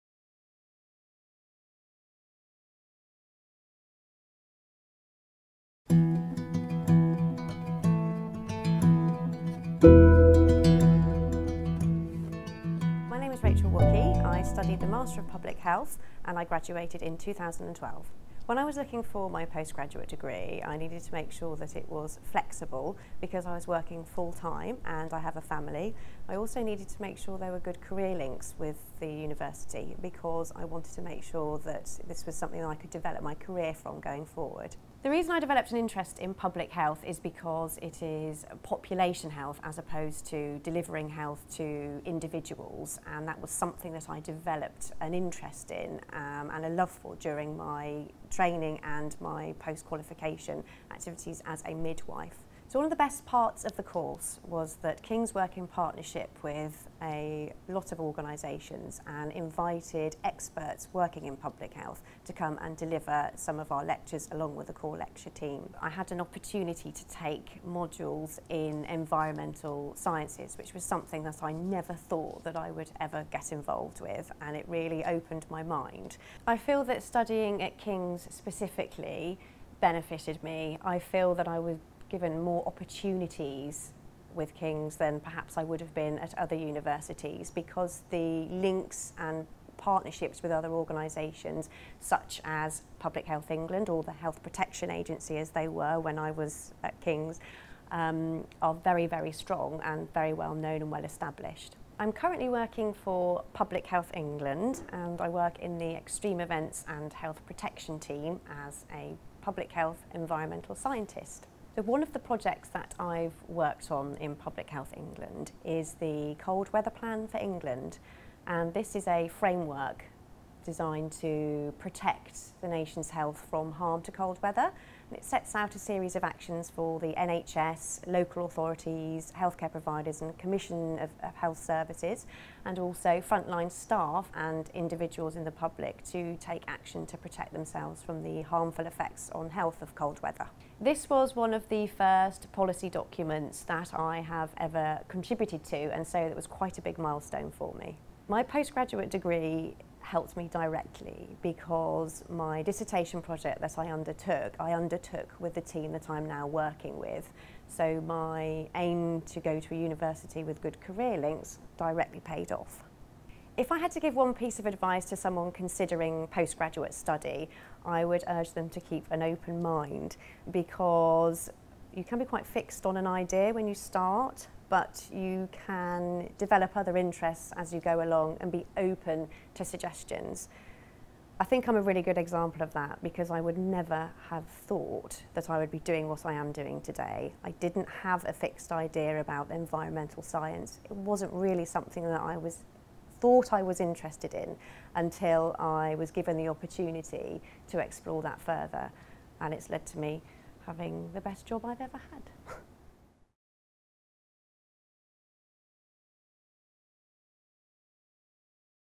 Rachel, Public Health MSc
Rachel completed her Public Health MSc at King's and now works as a scientist for Public Health England. Watch the video to learn more about her experience as a postgraduate student at King's. For more information about Postgraduate programmes at King's